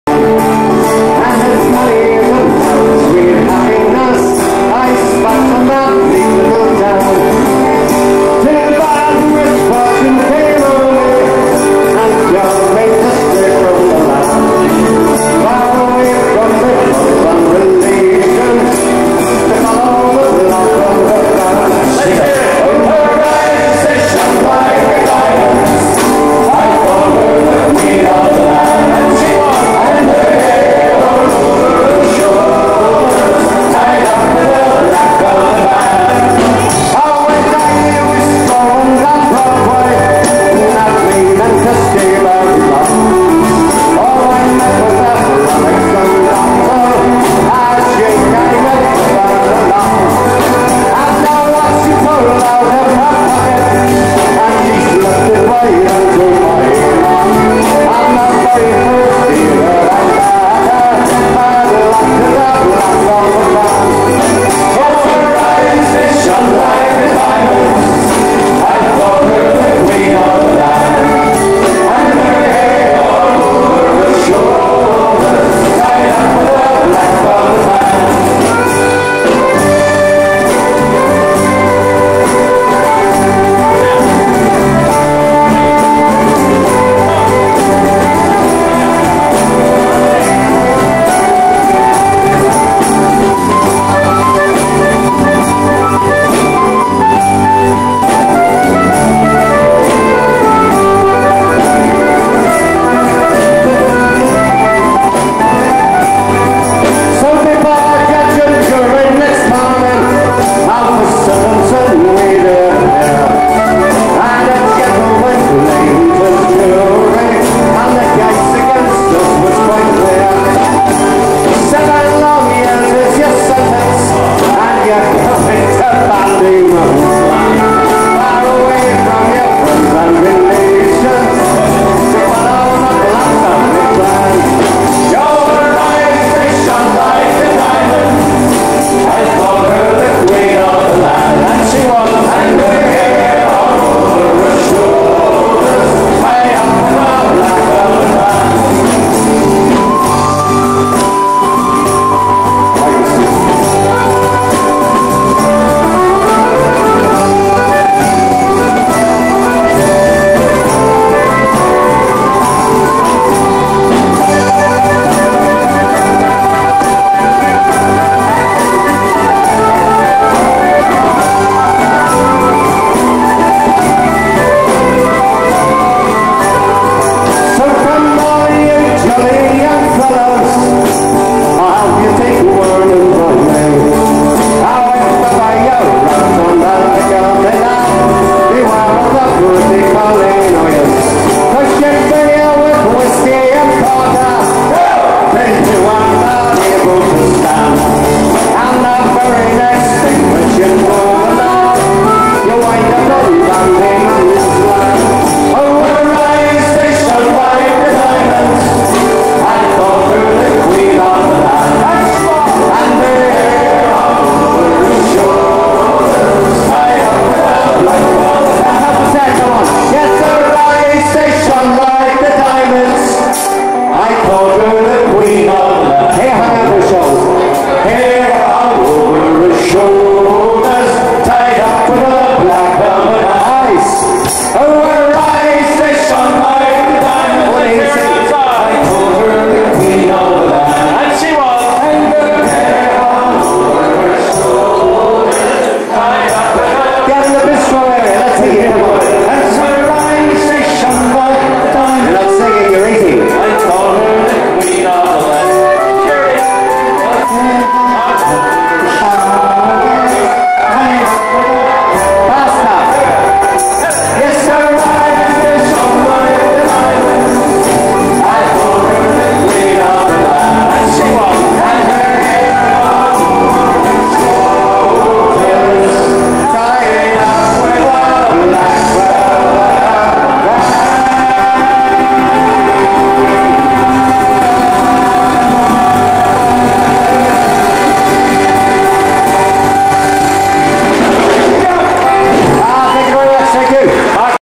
Murphy's pigs tamworth festival 18 jan 2015 at joe Macguires pub 1894
tamworth country festival at joe macguires pub 18th jan 2015 with Murphy's pigs Irish band doing Irish folk songs and australian folk music at joe Maguires Pub
148 Peel Street, Tamworth NSW 2340, Australia